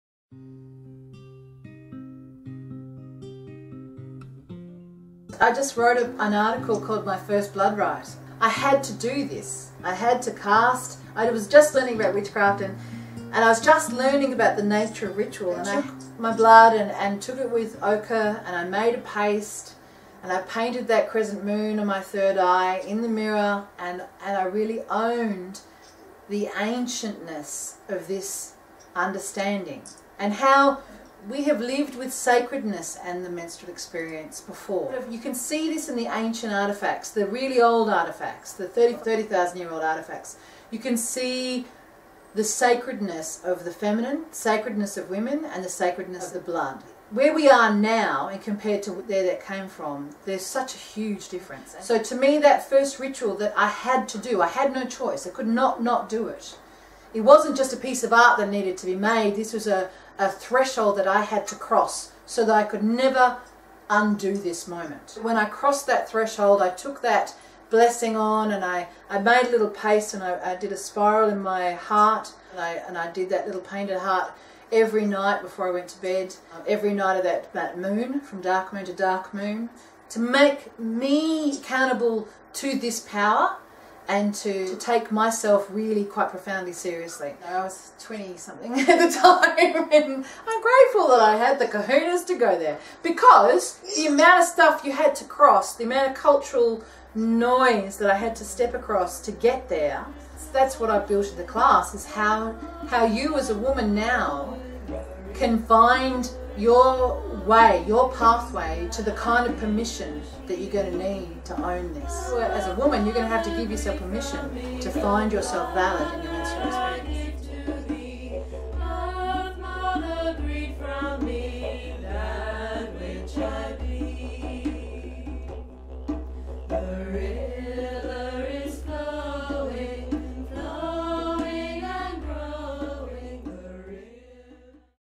Entering The Blood Work
This conversation is about how I began, how did I become a Menstrual Educator. It's about my first blood rite and the becoming of a blood witch. There is a blog post to link to when it becomes available.

StarFire is the work that comes from this intent. A circle technology that works best in your circle of women!

To read that whole article that I wrote please check out...